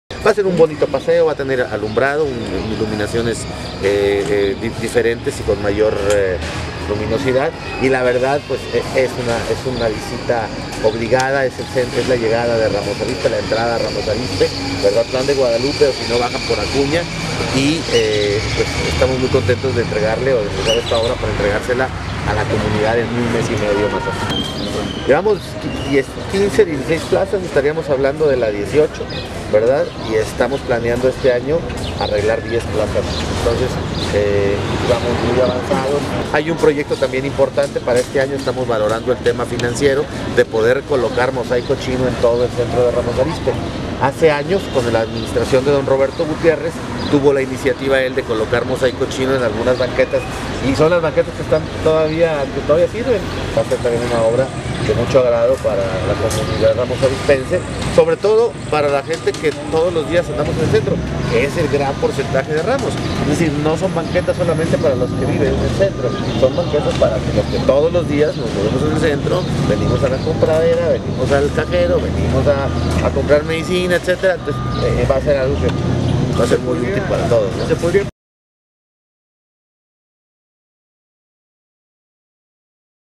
PONE EN MARCHA RICARDO AGUIRRE REHABILITACIÓN DE PLAZA
El alcalde de Ramos Arizpe, Ricardo Aguirre​, puso en marcha los trabajos de rehabilitación de la plaza Benito Juárez en el Centro de la ciudad.